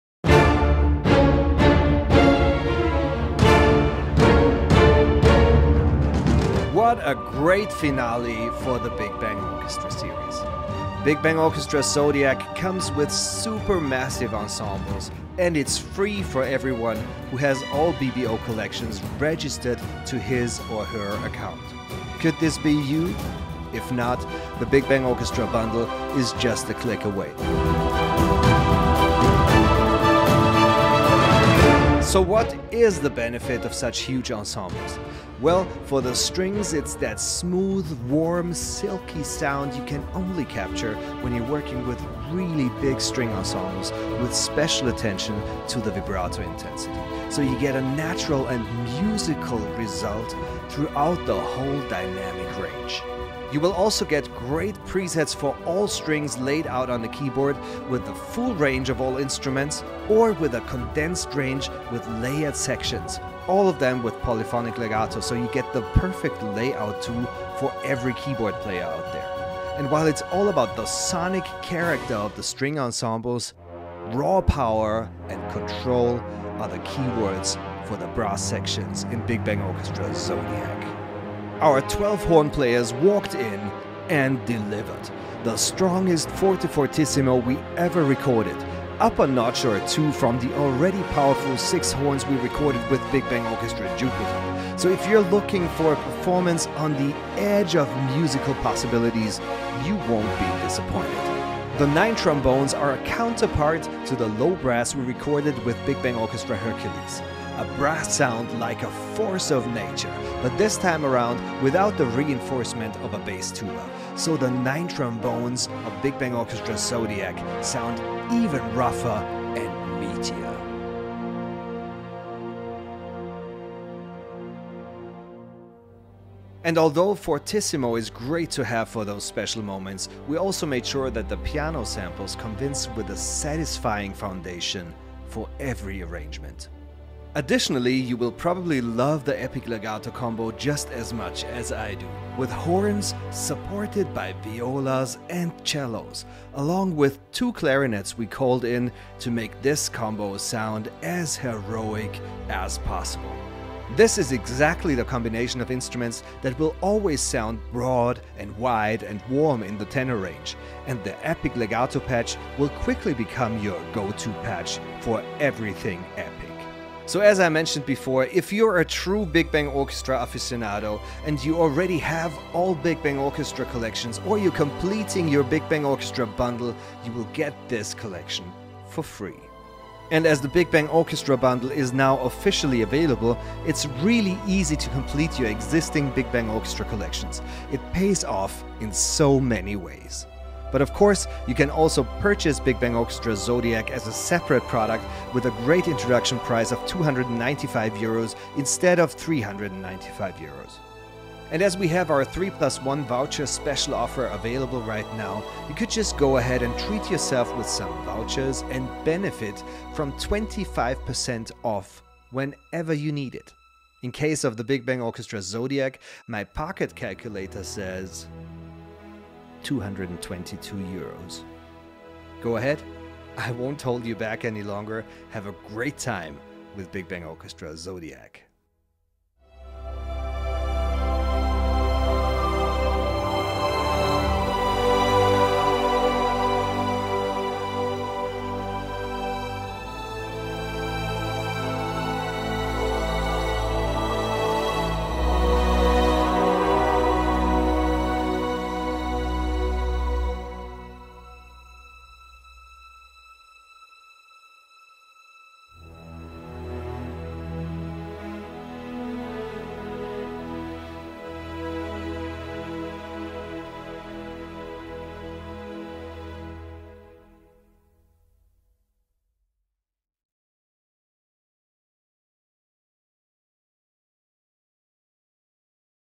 NEW BBO: Zodiac - Supermassive Ensembles - Introduction
It has been an excellent year for the growing Big Bang Orchestra Community! 25 (!!) Big Bang Orchestra Packages have shown a fresh approach to virtual orchestration, and now the super-sized ensembles in Big Bang Orchestra: Zodiac celebrate a grand finale for this amazing collection!

SAVE BIG with the new Bundle Pricing and get BBO: Zodiac for free as a bundle owner!

0:00 Intro
0:06 Introducing BBO: Zodiac
0:32 Strings
1:09 Brass
2:06 Dynamics
2:17 Epic Legato Combo
2:47 BBO Bundle & free BBO:Z
3:21 3+1 Voucher Special Offer
3:49 Outro & Demo